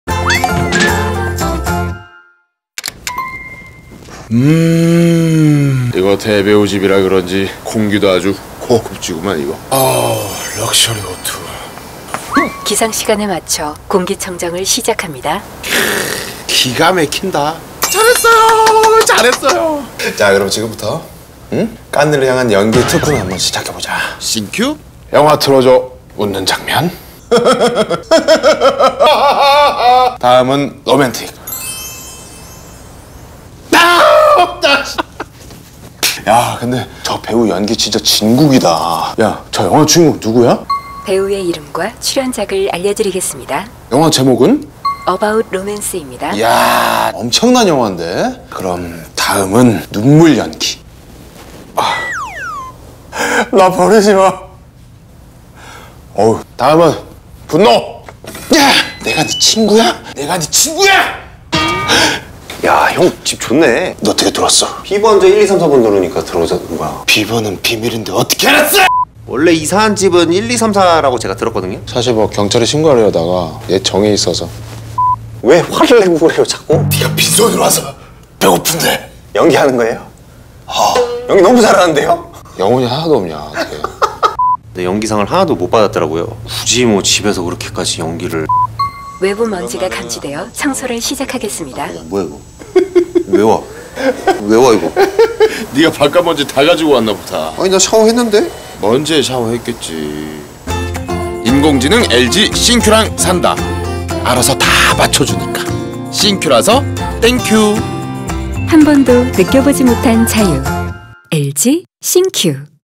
LG ThinQ - 나 혼자 안 산다 인공지능 LG ThinQ와 산다 이시언 편
나 혼자 안 산다
인공지능 LG ThinQ와 산다

어리~바리~했던 얼간이 라이프의 대표 3얼들의 유쾌한 변신!
나에게 맞춰 매일매일 새로워지는 인공지능 LG ThinQ 덕분에
더 쉽고 더 편해진 세 얼간이의 일상을 만나보세요.

▶'씽큐라서 땡큐' 인스타그램 빙고이벤트 참여하러가기